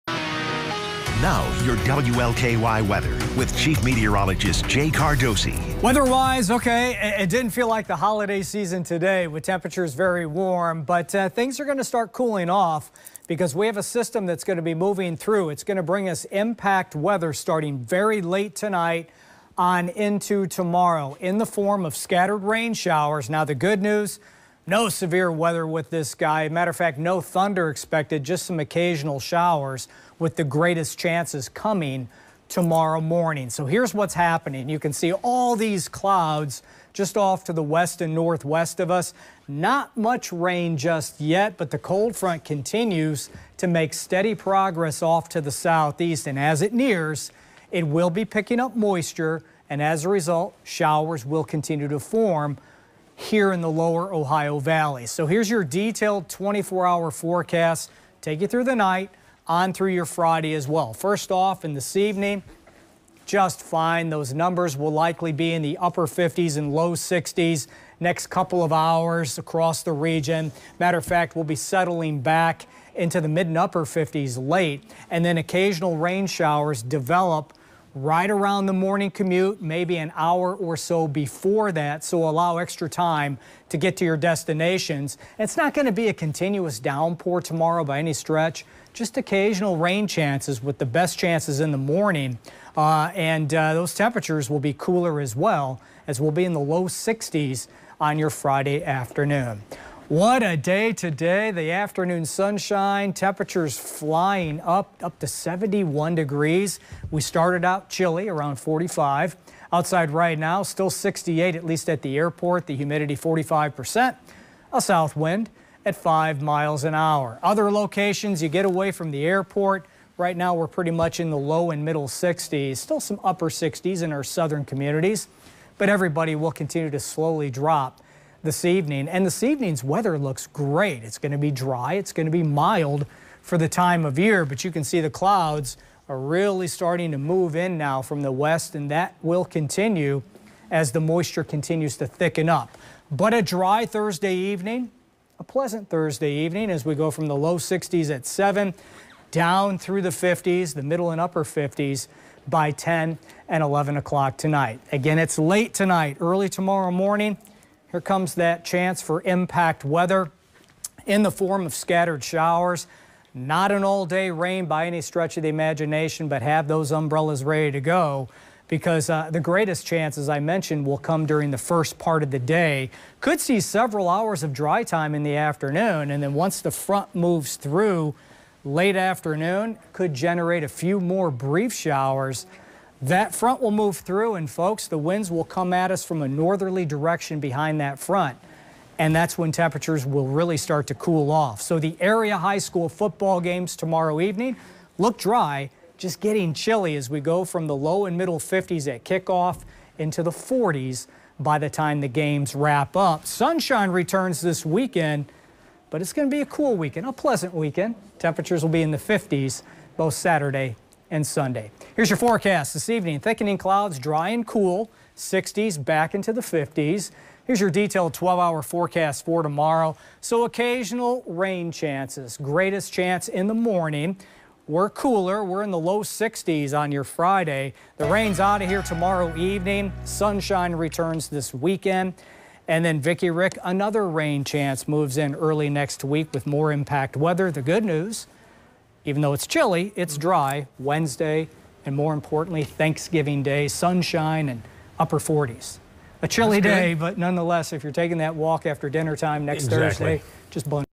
Impact weather for Friday followed by a cool weekend
Chief meteorologist Jay Cardosi talks about scattered showers for tomorrow followed by a sunny but cool November weekend.